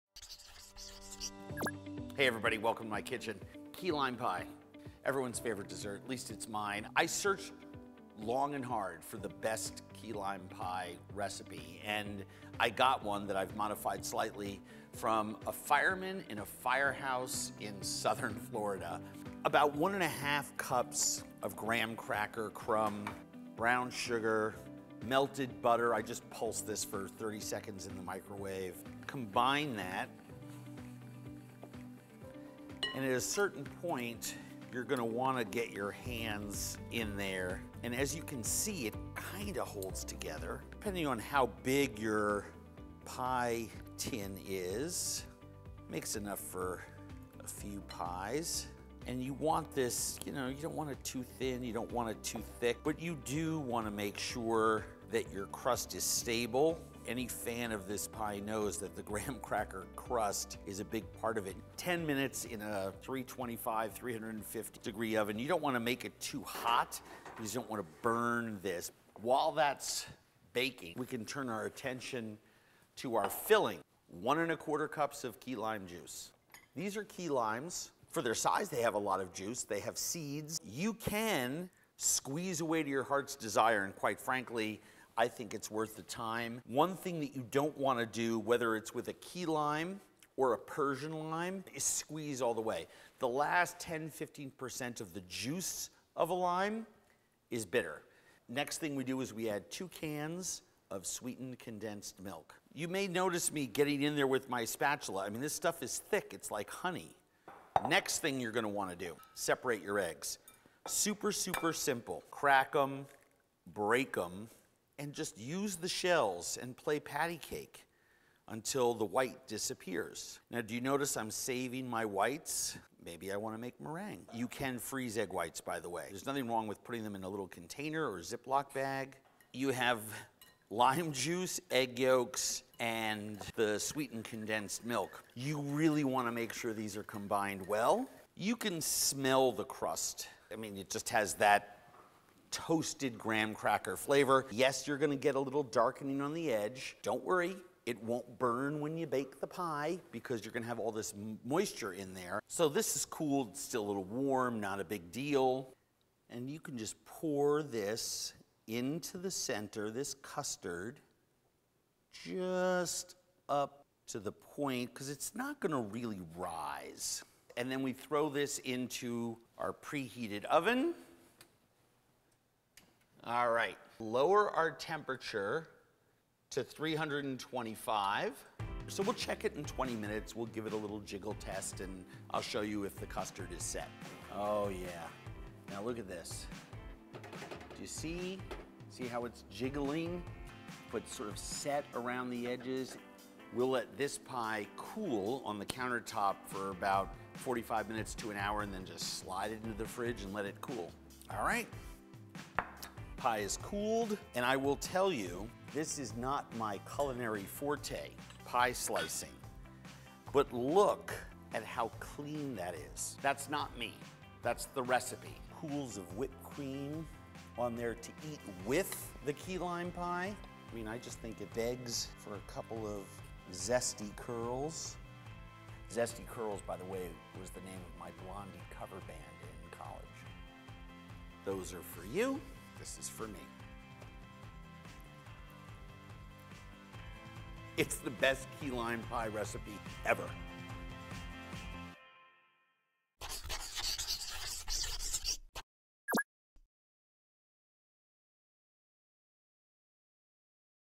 Andrew Zimmern Cooks: Key Lime Pie
I searched long and hard for the perfect Key lime pie recipe, and finally got THE ONE from a Miami fireman while filming in Florida years ago. The ratios are spot on and the custard turns out perfectly every time. Make this recipe with lemons, Meyer lemons, grapefruits, sour oranges… any citrus that is super tart. This bright, bold and cold pie is on my mind all summer long, its citrus flavor is the best palate cleanser.